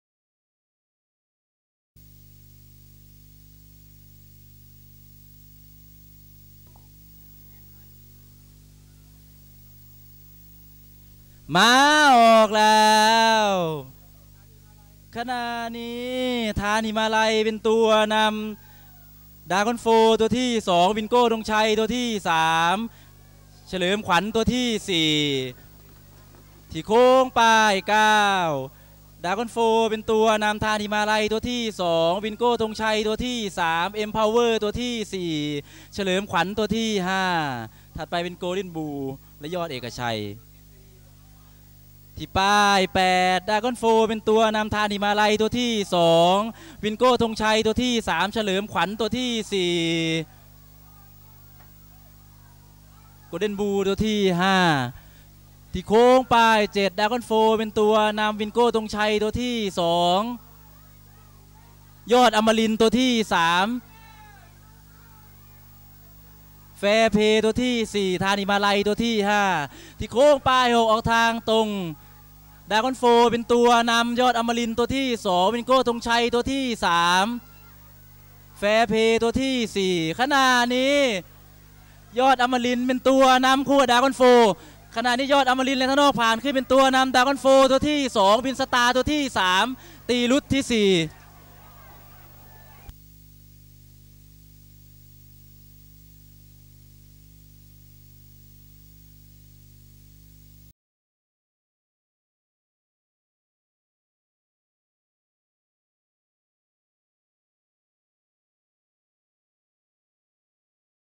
ม้าแข่งสนามโคราช 2 ก.ค. 60 เที่ยว 6 ม้าเทศชั้น 3
ผลการแข่งม้าเมื่อวันอาทิตย์ที่ 2 กรกฎาคม 2560
(ครั้งที่ 25/60)  องค์การกองทัพภาคที่ 2
ระยะ 31.5 เส้น สภาพสนามดี
ขอบคุณ นายสนาม พันเอกถกล มักการุณ